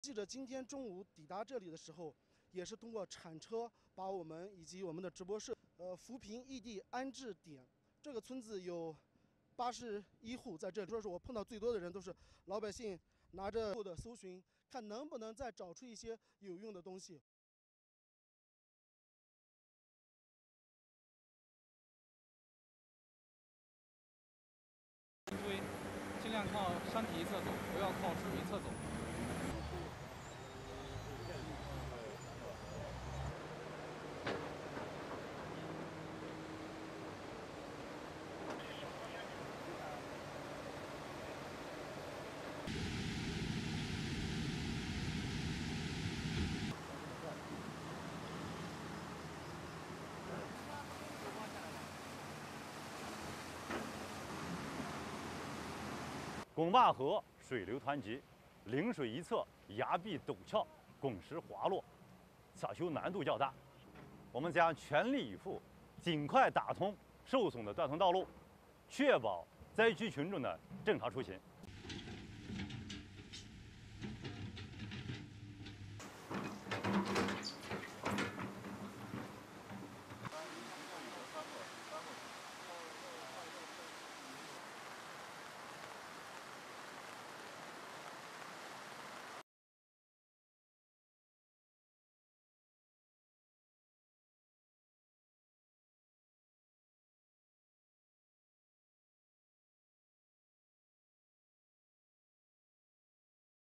Soldiers Racing Against Time to Resume Traffic on Roads Damaged in Northwest China Landslides
Soldiers from local Chinese People's Liberation Army (PLA) and Chinese Armed Police units are racing against time to repair the roads damaged in landslides in Zhouqu County, northwest China's Gansu Province.